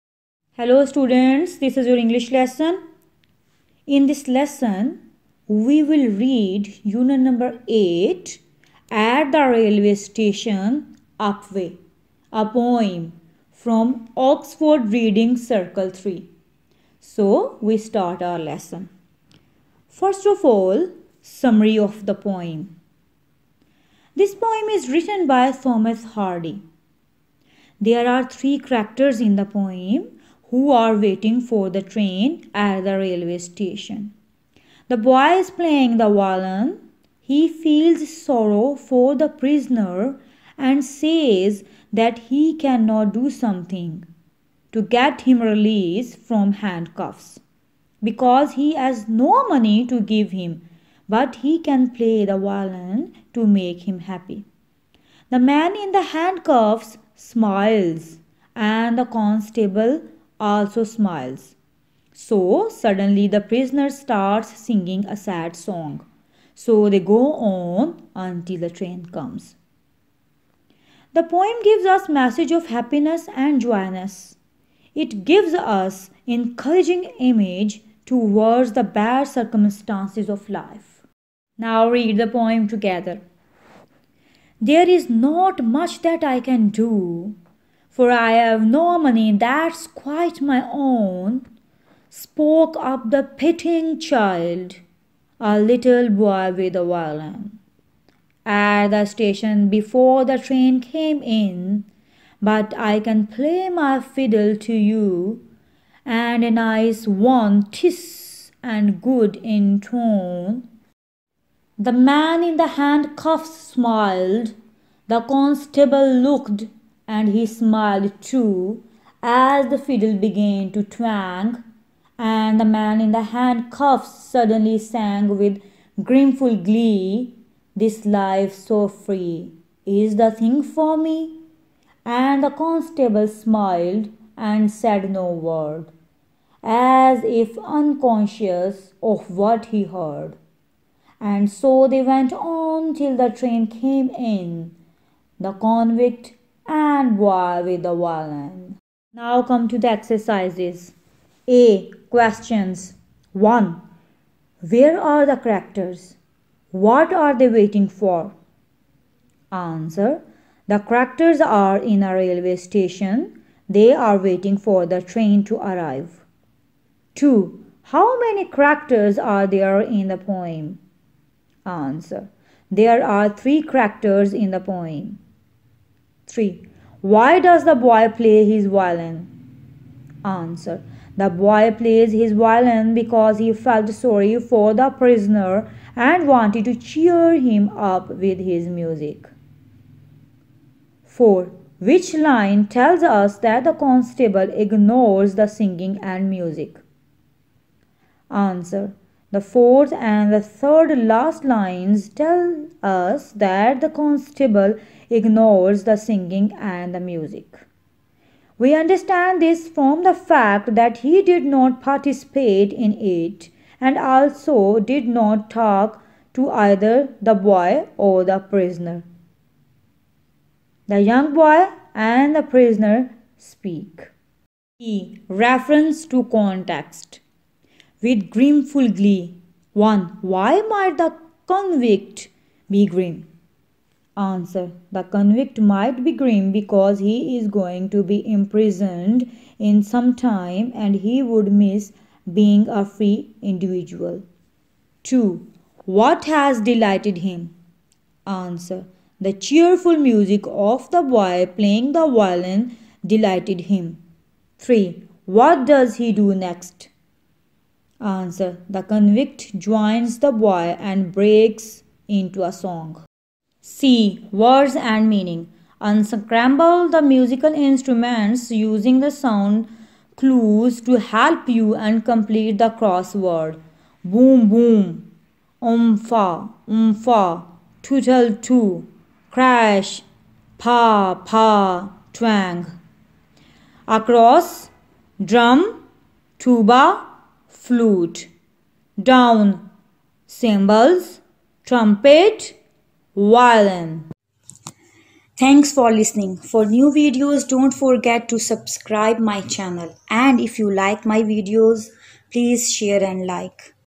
Unit 8 # At the Railway Station, Up way—Thomas Hardy (Oxford Reading circle grade 3 2nd Edition)
Hello Students,

In this video you will get lecture of Unit 8 # At the Railway Station, Up way—Thomas Hardy  from (Oxford Reading circle grade 3 2nd Edition Nicholas Horsburgh & Claire Horsburgh) Reading & Questions / Answers. I hope you will understand this lesson.